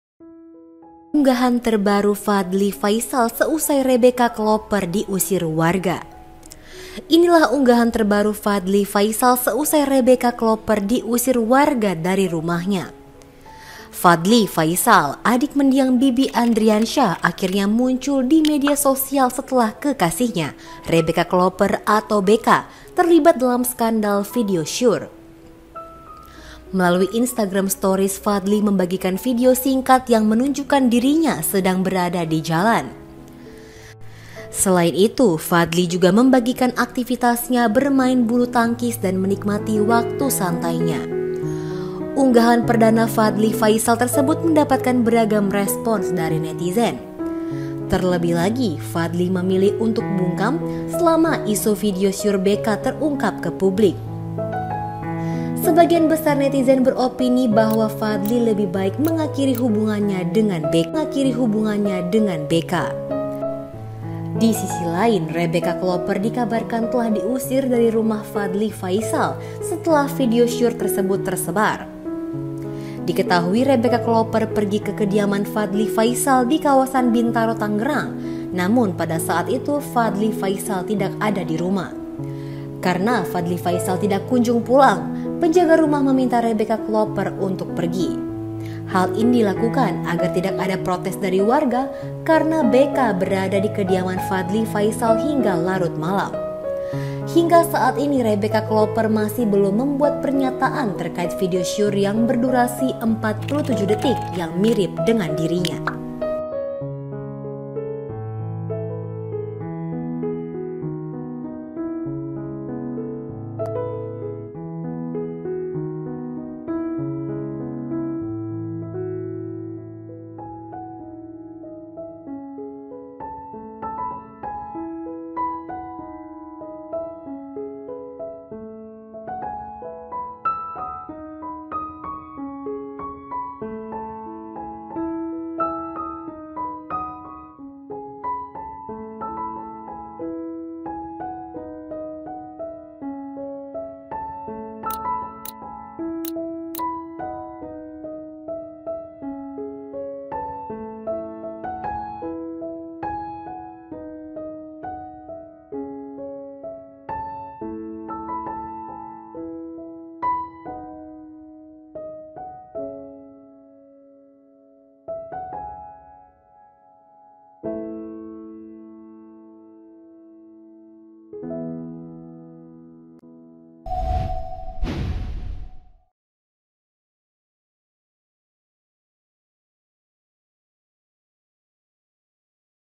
Unggahan Fadly Faisal Seusai Rebecca Klopper Diusir Warga
TRIBUNBATAM.ID - Inilah unggahan terbaru Fadly Faisal seusai Rebecca Klopper diusir warga dari rumahnya.

Melalui Instagram Stories, Fadly membagikan video singkat yang menunjukkan dirinya sedang berada di jalan.

Selain itu, Fadly juga membagikan aktivitasnya bermain bulu tangkis dan menikmati waktu santainya.

Unggahan perdana Fadly Faisal tersebut mendapatkan beragam respons dari netizen.

Terlebih lagi, Fadly memilih untuk bungkam selama isu video syur Becca terungkap ke publik.

Sebagian besar netizen beropini bahwa Fadly lebih baik mengakhiri hubungannya dengan Becca.

Di sisi lain, Rebecca Klopper dikabarkan telah diusir dari rumah Fadly Faisal setelah video syur tersebut tersebar.

Diketahui, Rebecca Klopper pergi ke kediaman Fadly Faisal di kawasan Bintaro, Tangerang, namun pada saat itu Fadly Faisal tidak berada di rumah.

Karena Fadly Faisal tidak kunjung pulang, penjaga rumah meminta Rebecca Klopper untuk pergi.

Hal ini dilakukan agar tidak ada protes dari warga karena Rebecca Klopper berada di kediaman Fadly Faisal hingga larut malam.

Hingga saat ini, Rebecca Klopper masih belum membuat pernyataan terkait video syur yang berdurasi 47 detik yang mirip dengan dirinya.

VE:MD